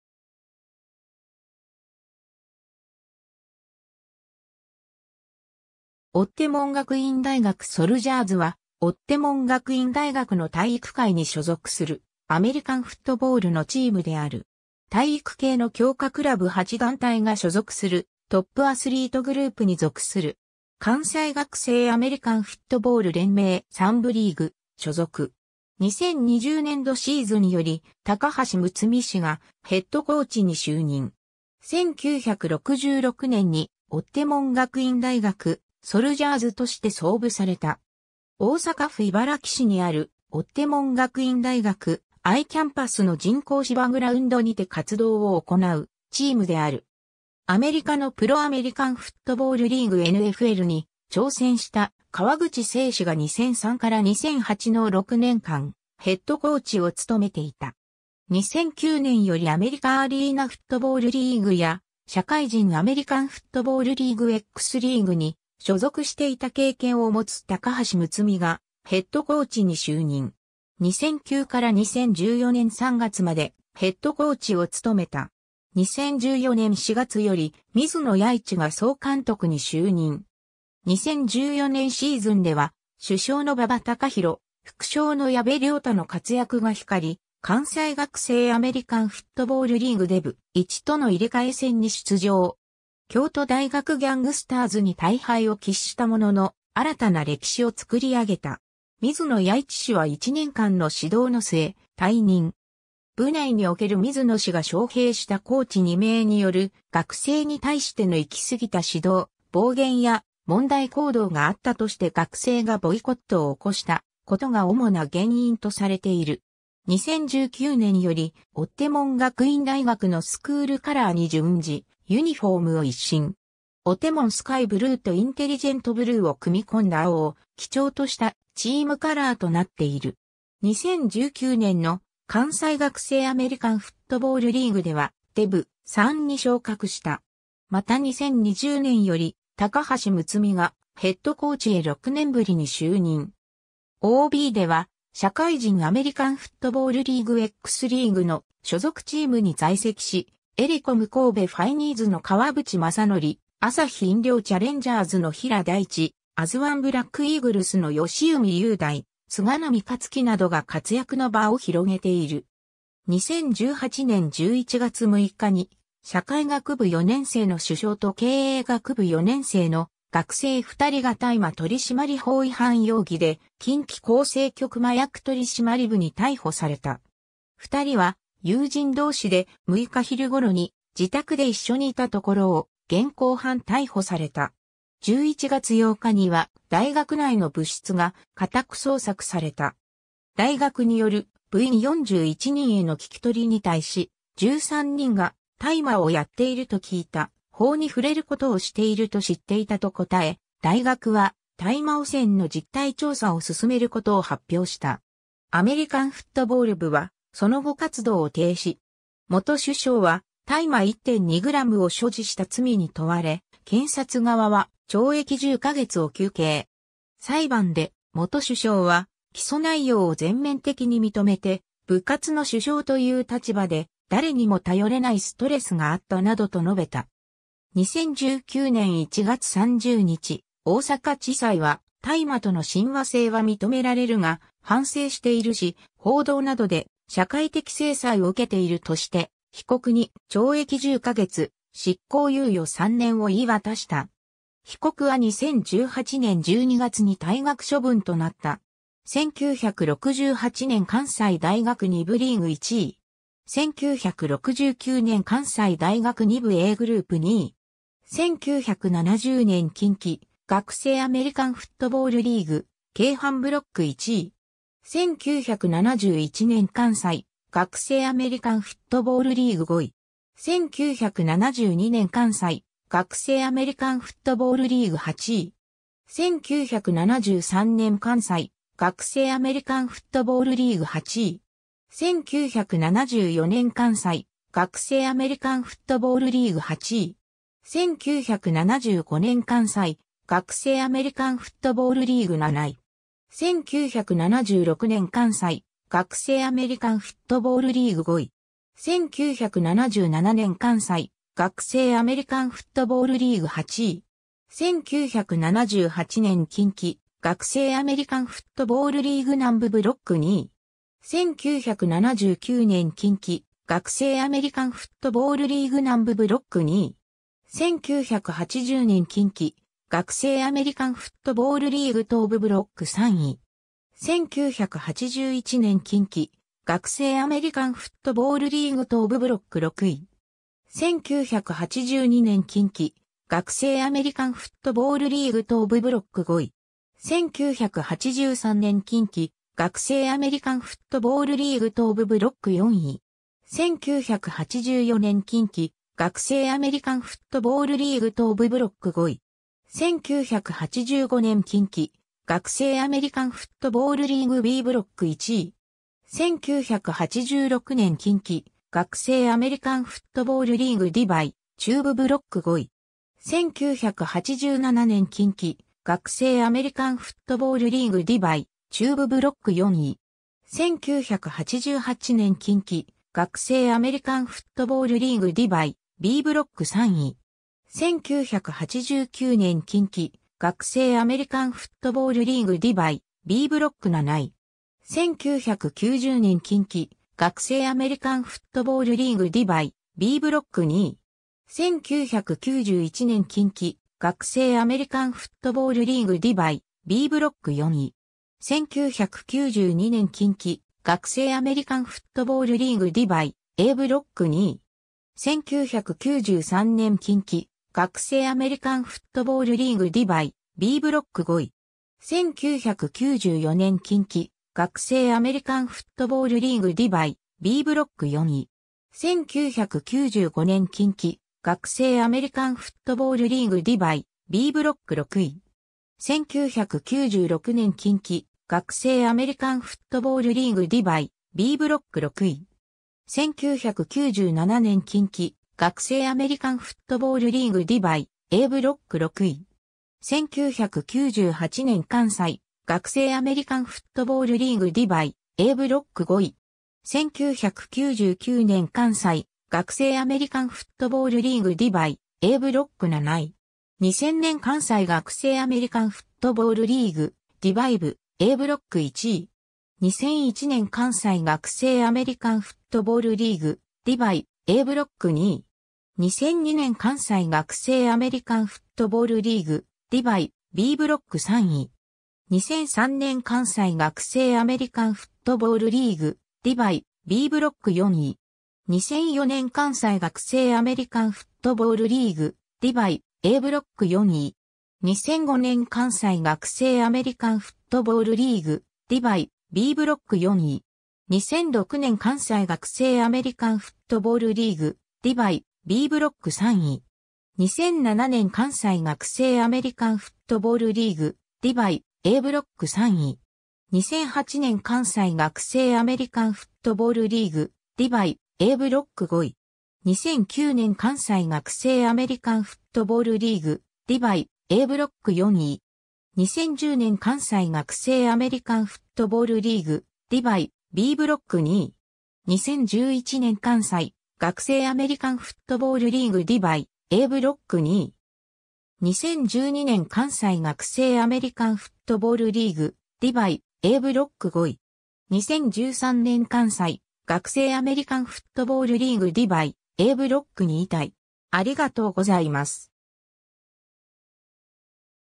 追手門学院大学ソルジャーズ
追手門学院大学ソルジャーズ（おうてもんがくいんだいがくソルジャーズ）は、追手門学院大学の体育会に所属するアメリカンフットボールのチームである。
体育系の強化クラブ8団体が所属するトップアスリートグループに属する。
関西学生アメリカンフットボール連盟3部リーグ所属。
 2020年度シーズンより高橋睦巳氏がヘッドコーチに就任。
 1966年に追手門学院大学ソルジャーズとして創部された。
大阪府茨木市にある追手門学院大学安威キャンパスの人工芝グラウンドにて活動を行うチームである。
 アメリカのプロアメリカンフットボールリーグNFLに挑戦した河口正史が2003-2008の6年間ヘッドコーチを務めていた。
 2009年よりアメリカアリーナフットボールリーグや社会人アメリカンフットボールリーグXリーグに所属していた経験を持つ高橋睦巳がヘッドコーチに就任。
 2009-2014年3月までヘッドコーチを務めた。
 2014年4月より水野彌一が総監督に就任。
 2014年シーズンでは主将の馬場貴弘、副将の矢部椋太の活躍が光り、関西学生アメリカンフットボールリーグDiv.1との入替戦に出場。
京都大学ギャングスターズに大敗を喫したものの新たな歴史を創り上げた。
 水野彌一氏は１年間の指導の末、退任。
部内における水野氏が招聘したコーチ２名による学生に対しての行き過ぎた指導、暴言や問題行動があったとして学生がボイコットを起こした事が主な原因とされている。
 2019年より追手門学院大学のスクールカラーに準じ、ユニフォームを一新。
OTEMON SKY BLUEとINTELLIGENT BLUEを組み込んだ青を貴重としたチームカラーとなっている。
 2019年の関西学生アメリカンフットボールリーグではDiv.3に昇格した。
 また2020年より高橋睦巳がヘッドコーチへ6年振りに就任。
 OBでは社会人アメリカンフットボールリーグXリーグの所属チームに在籍し、エレコム神戸ファイニーズの川淵将紀、アサヒ飲料チャレンジャーズの平大地、アズワンブラックイーグルスの吉海雄大、菅波克規などが活躍の場を広げている。
 2018年11月6日に社会学部4年生の主将と経営学部4年生の学生2人が大麻取締法違反（所持）容疑で近畿厚生局麻薬取締部に逮捕された。
2人は友人同士で、6日昼頃に自宅で一緒にいたところを現行犯逮捕された。
11月8日には大学内の部室が家宅捜索された。
大学による部員41人への聞き取りに対し、13人が「（主将が）大麻をやっていると聞いた」、「法に触れることをしていると知っていた」と答え、大学は大麻汚染の実態調査を進めることを発表した。
アメリカンフットボール部はその後活動を停止。
 元主将は、大麻1.2グラムを所持した罪に問われ、検察側は懲役10ヵ月を求刑。
裁判で元主将は、起訴内容を全面的に認めて「部活の主将という立場で、誰にも頼れないストレスがあった」などと述べた。
2019年1月30日、大阪地裁は「大麻との親和性は認められるが、反省しているし、報道などで社会的制裁を受けている」として、被告に懲役10ヵ月、執行猶予3年を言い渡した。
被告は2018年12月に退学処分となった。
 1968年(S43年)　関西大学2部リーグ  　　　　 　　　　　　　　　　 　 1位(2部リーグ新設、初参戦)
1969年(S44年)　関西大学2部Aグループ  　　　　 　　　　　　　　　　 2位
1970年(S45年)　近畿学生アメリカンフットボールリーグ京阪ブロック  　1位(入れ替え戦出場、1部昇格)
1971年(S46年)　関西学生アメリカンフットボールリーグ(1部)  　　　　 5位
1972年(S47年)　関西学生アメリカンフットボールリーグ(1部)  　　　　 8位(入れ替え戦出場、1部残留)
1973年(S48年)　関西学生アメリカンフットボールリーグ(1部)  　　　　 8位(入れ替え戦出場、1部残留)
1974年(S49年)　関西学生アメリカンフットボールリーグ(1部)  　　　　 8位(入れ替え戦出場、1部残留)
1975年(S50年)　関西学生アメリカンフットボールリーグ(1部)  　　　　 7位(入れ替え戦出場、1部残留)
1976年(S51年)　関西学生アメリカンフットボールリーグ(1部)  　　　　 5位
1977年(S52年)　...